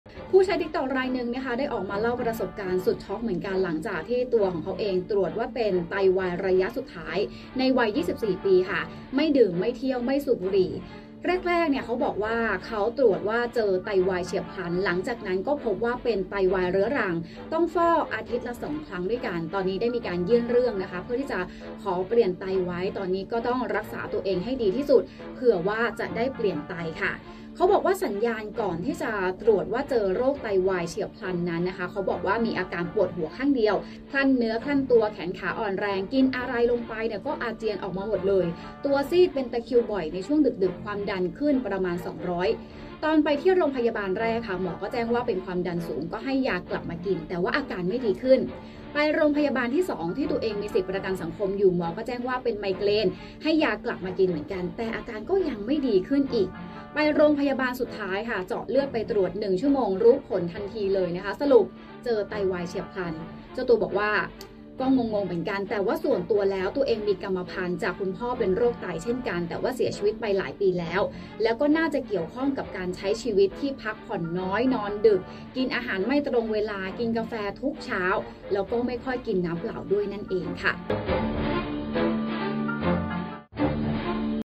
หนุ่มวัย 24 ปี ตรวจเจอไตวายระยะสุดท้าย ทั้งที่ไม่ดื่ม ไม่เที่ยว ไม่สูบบุหรี่ | Thainews - ไทยนิวส์
หนุ่มวัย 24 ปี ตรวจเจอไตวายระยะสุดท้าย ทั้งที่ไม่ดื่ม ไม่เที่ยว ไม่สูบบุหรี่ ก่อนเผยสาเหตุแท้จริง แห่ให้กำลังใจสนั่น